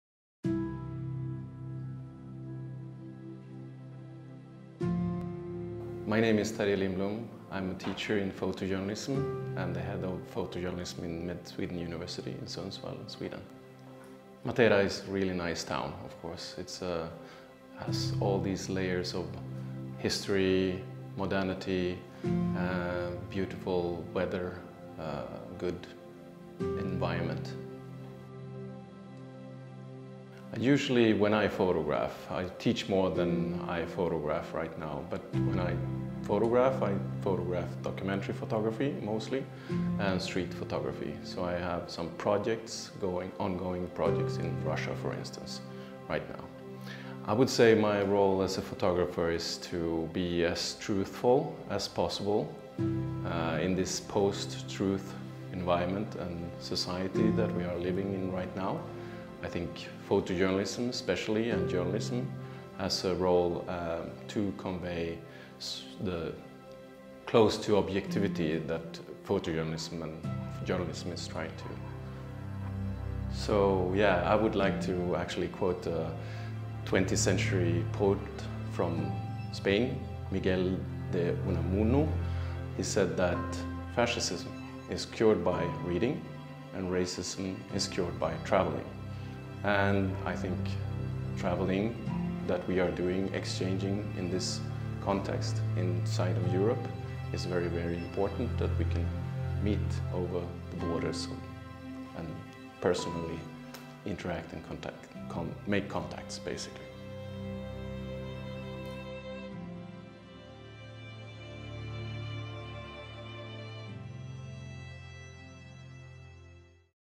Terje Lindblom - Sweden
I'm the head of photojournalism at Mid-Sweden university. My background is an extensive newspaper career as a photo editor, journalist and photojournalist as well as I have ten years of experience as a teacher in the subject of photojournalism, both on bachelor and international master level in higher education.
Currently, I’m writing a dissertation on the subject of professional changes and evelopments in photojournalism and to photojournalist in Swedish newspaper media field.